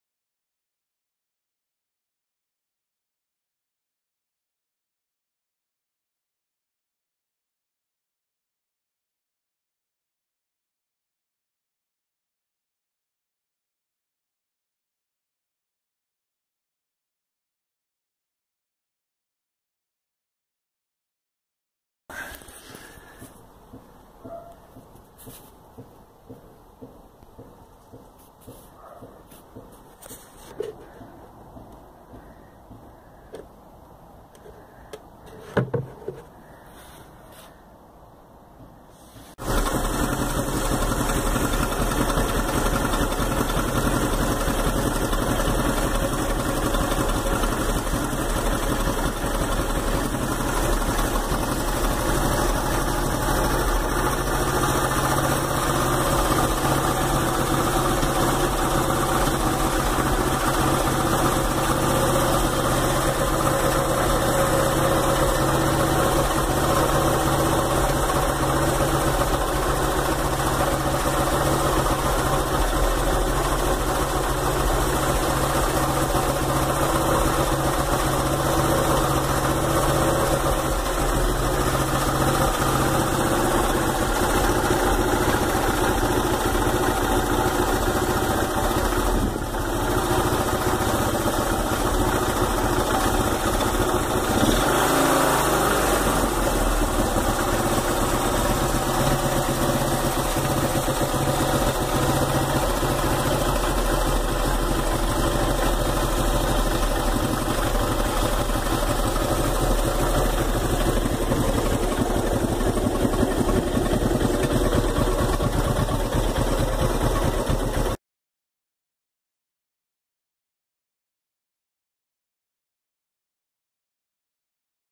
1972 Honda XL250 K0 Motosport
Next step in the rebuild, still along way to go